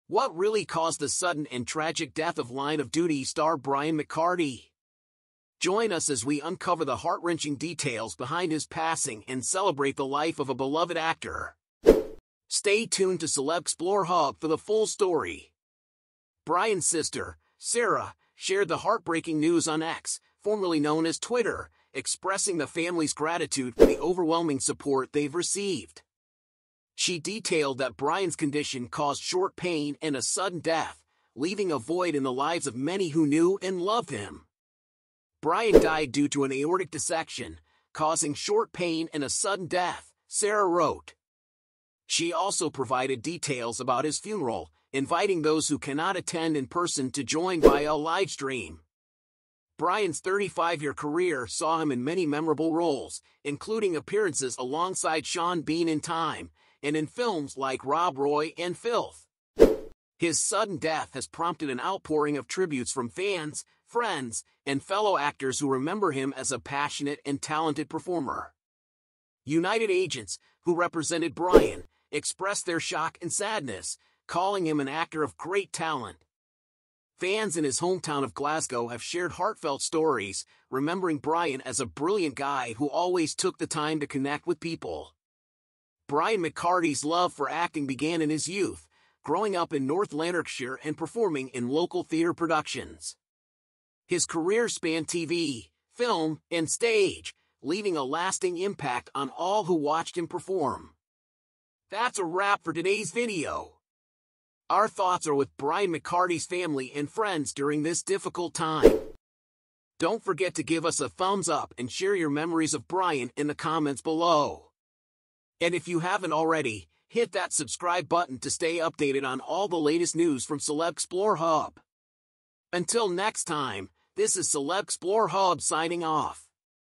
.Heartbreaking: Brian McCardie's Cause of Death Revealed by Family
"What really caused the sudden and tragic death of 'Line of Duty' star Brian McCardie? Join us as we uncover the heart-wrenching details behind his passing and celebrate the life of a beloved actor.